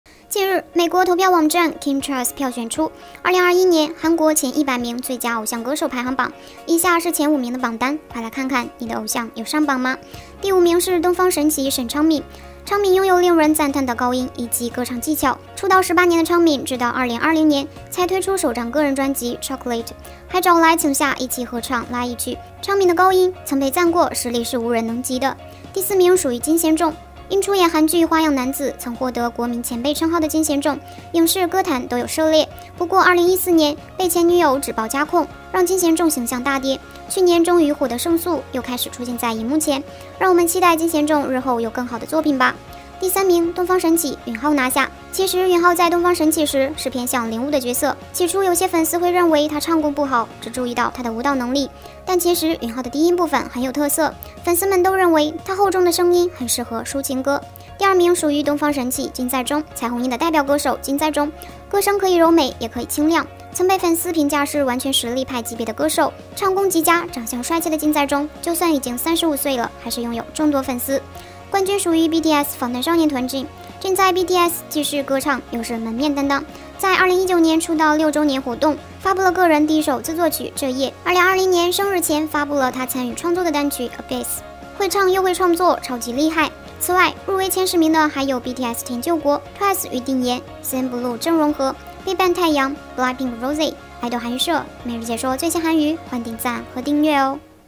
2021韓國最佳偶像歌手Top 10出爐！ BLACKPINK第十、TWICE第七，BTS佔2位！！
2021韓國最佳偶像歌手Top 10出爐！ BLACKPINK第十、TWICE第七，BTS佔2位！！